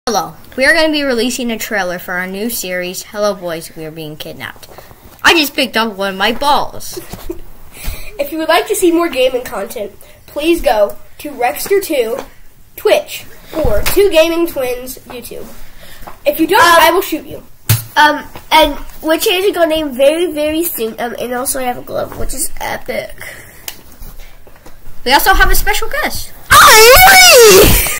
Trailer Coming Soon...
We will be releasing the trailer for our series, "Hello Boys, We Are Being Kidnapped"
Our series will also be released soon, most likely on Tuesday.